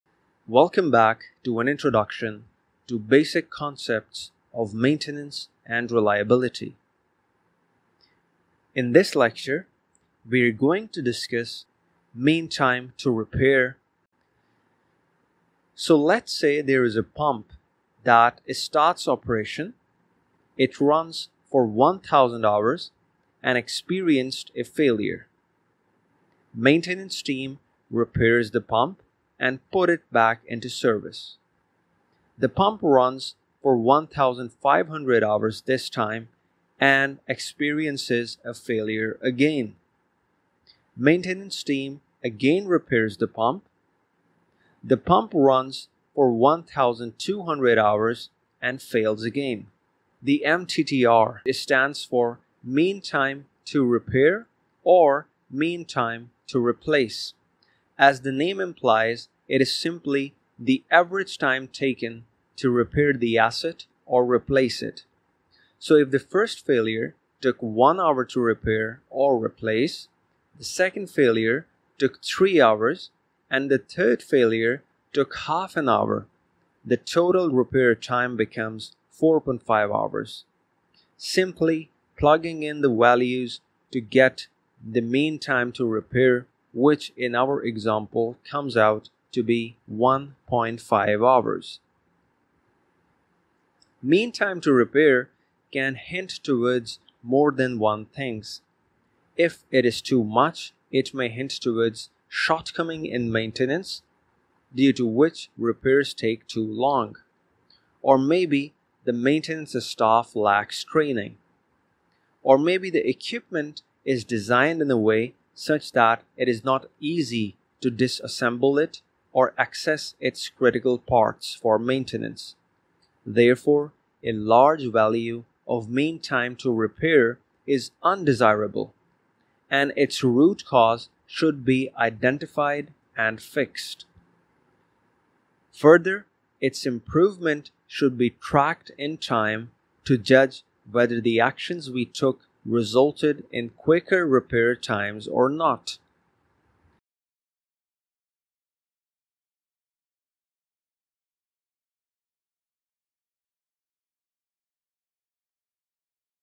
Reliability Metrics: MTTR | Mean Time To Repair | Mean Time To Replace | Introduction & Calculation
We explain an important metric used in maintenance management and reliability engineering i.e., the MTTR which stands for Mean Time to Repair or the Mean Time to Replace (depending upon the context).

This video is part of an online video based course on introducing the basic concepts of maintenance and reliability by following the CMRP body of knowledge. The course is focused on Pillar No. 3 (Equipment Reliability) of the referred body of knowledge.

Video Chapters
00:00 Intro
01:34 Interpretation